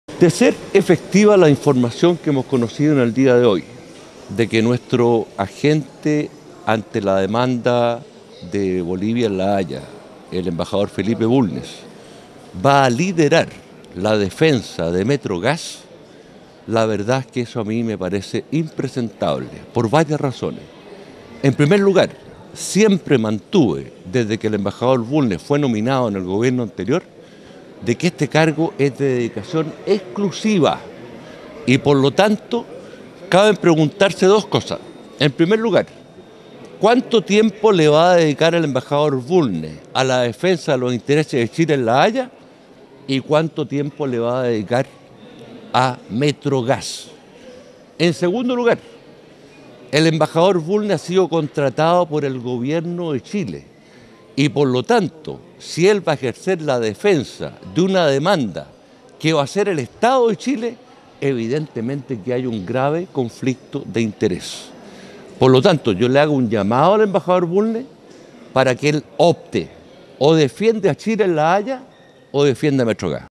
Diputado Tarud criticó a ex embajador Bulnes por representar a Metrogas
El parlamentario dijo que Bulnes debe optar por representar a Chile en La Haya o representar a Metrogas.